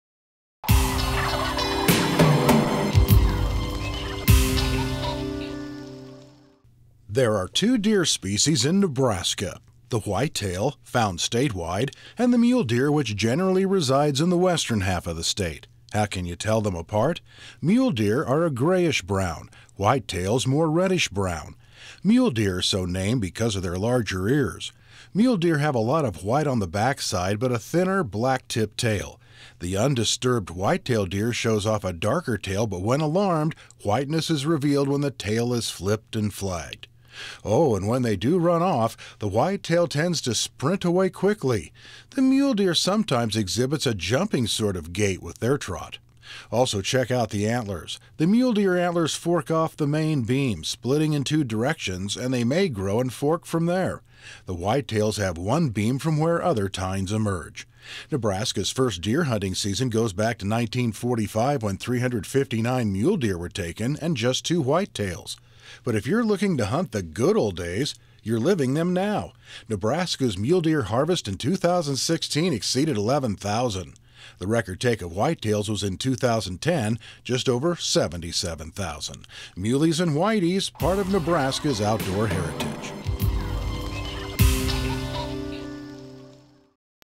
Nebraska Outdoor Heritage - Deer
A look at the differences between Mule Deer and Whitetail Deer and some historic Nebraska deer hunting figures.